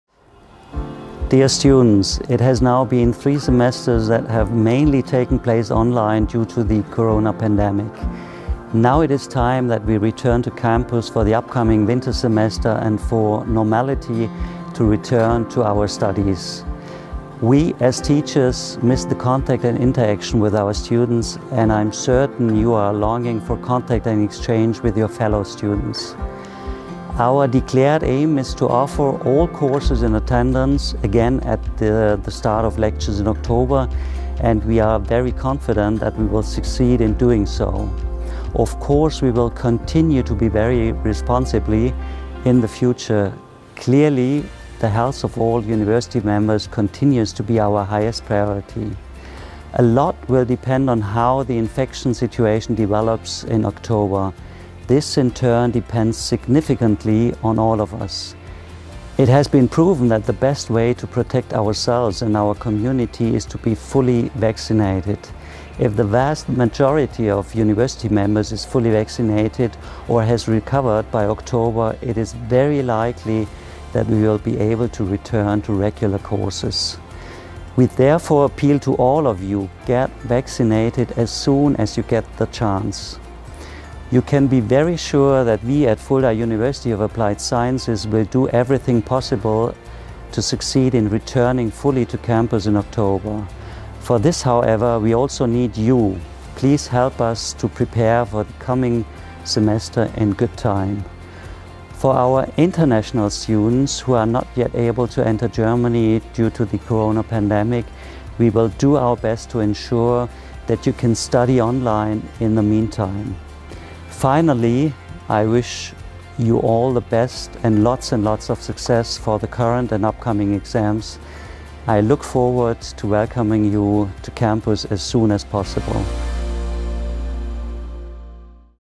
Video message from the President of Fulda University of Applied Sciences to the students
The President of Fulda University of Applied Sciences gives an outlook on the coming winter semester (2021/22). The aim is to offer all courses in attendance again at the start of lectures. He also wishes the students all the best for the current and upcoming exams.